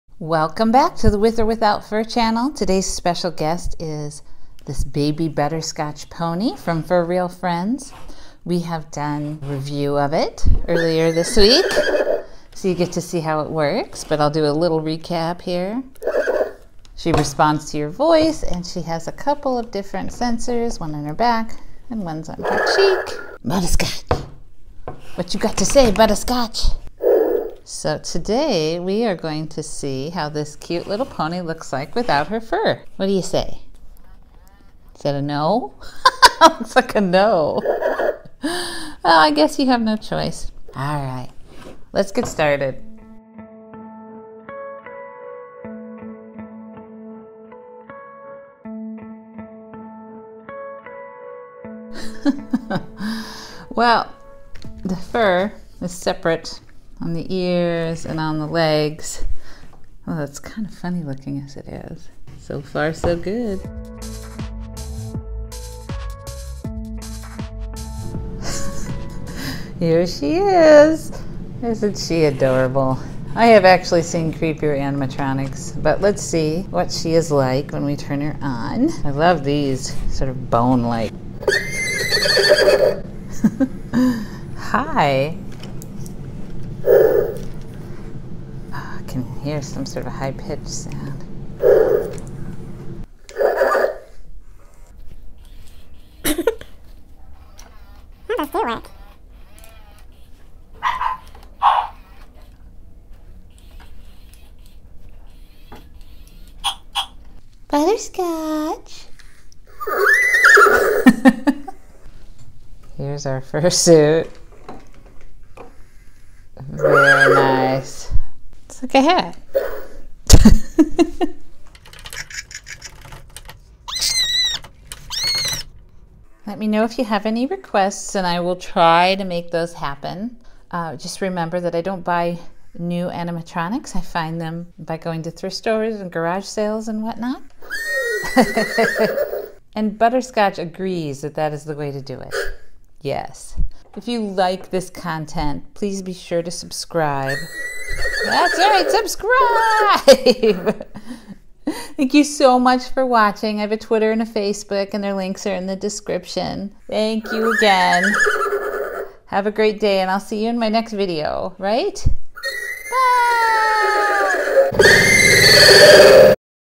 FurReal Friends Baby Butterscotch Pony Without Fur
Butterscotch was a lot of work to remove the fur.  I hope you enjoy it!

I love requests, but please be aware I cannot buy any toys brand new.  I peruse thrift stores for the animatronics in my videos.  I prefer they are well loved and used before they find their way to me.